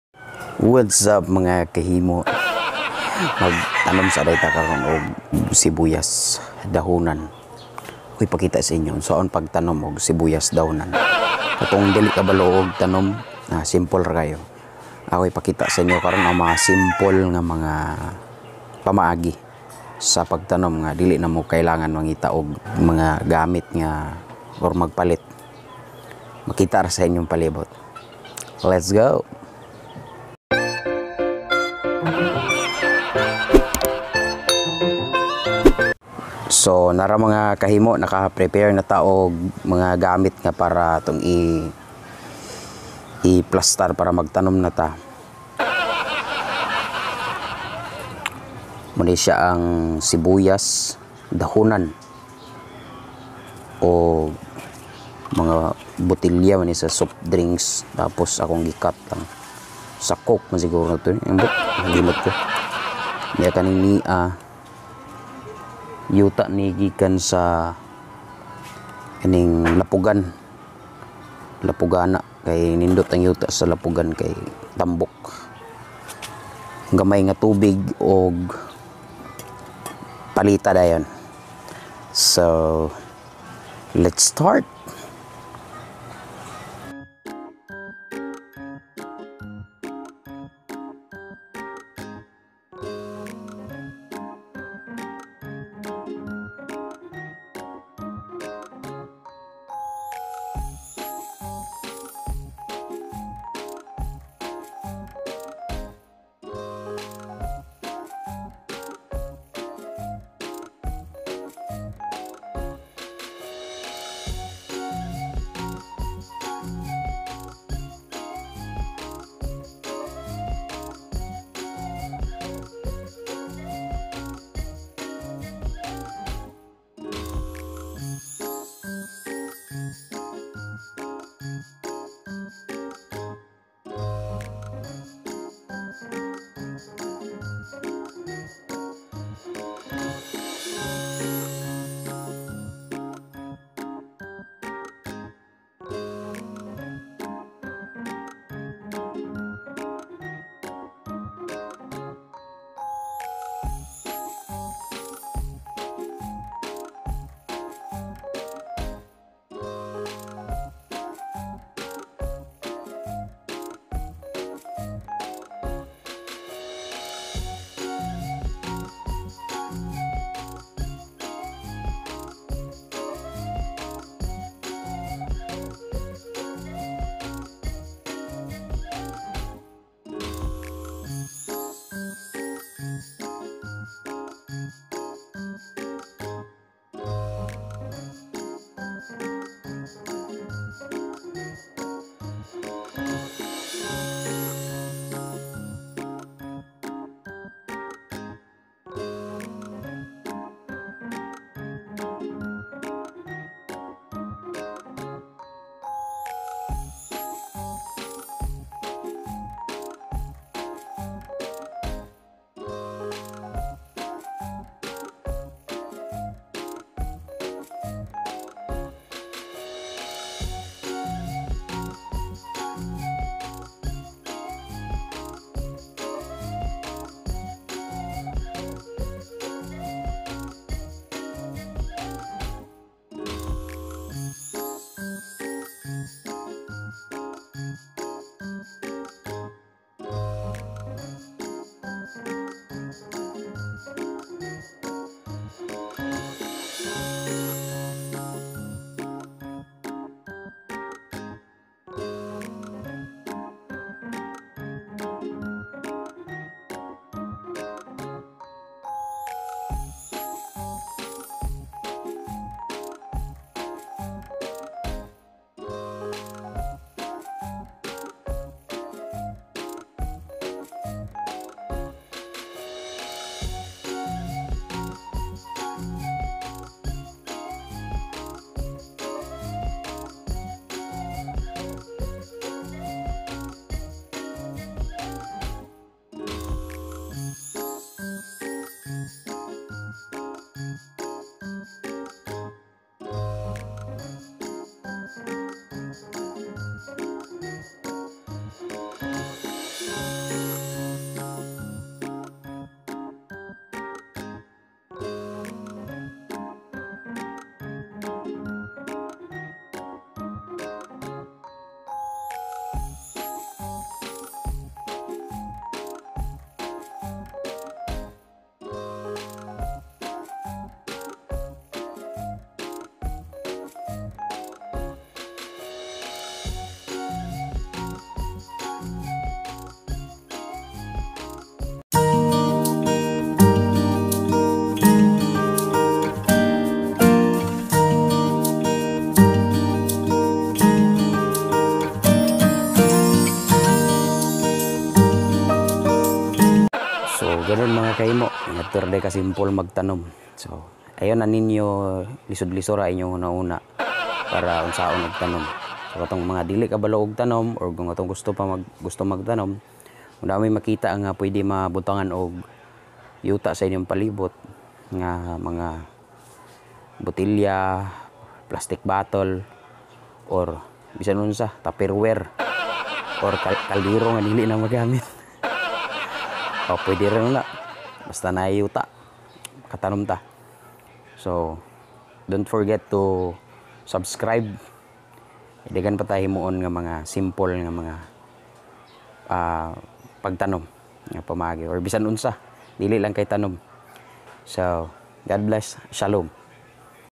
HOW TO GROW SPRING ONIONS AT HOME | IN SIMPLE STEPS | HARING DABID DIY
Wazzup mga kahimo😁